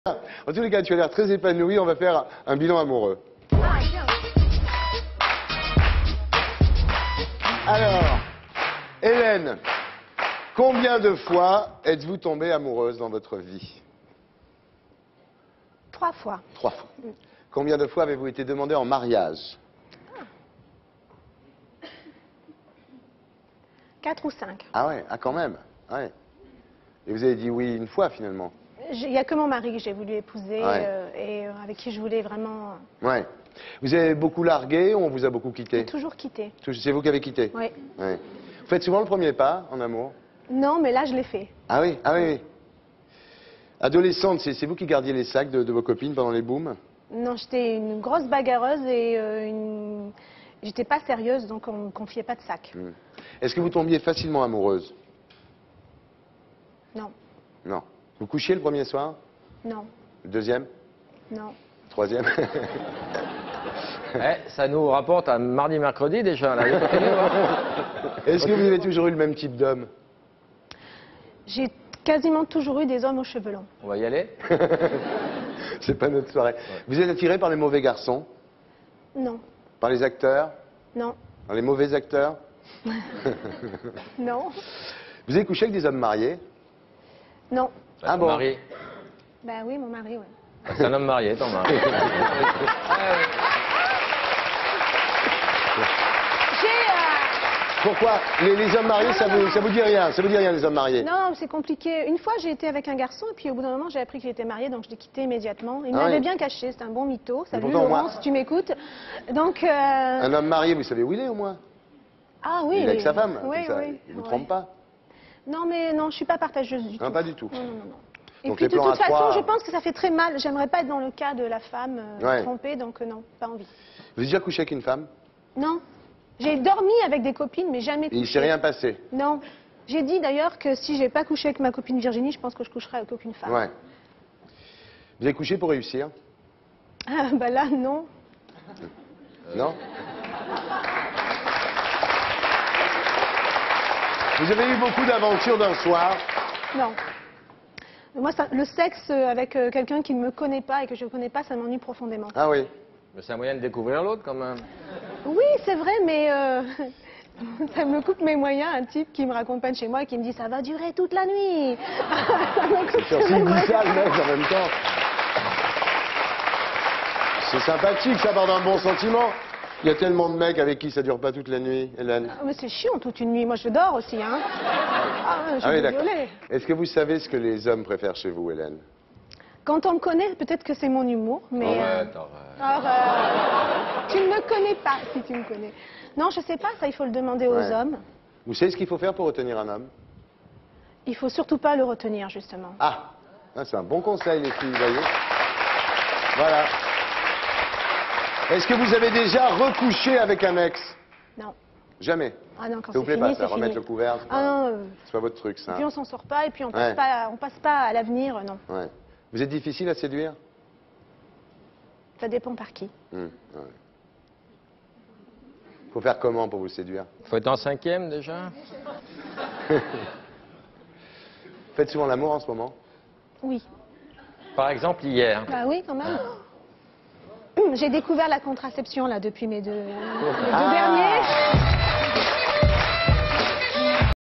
Bilan amoureux d'Hélène Segara - Archive INA
Questions de Thierry ARDISSON à Hélène SEGARA sur sa vie amoureuse. Images d'archive INA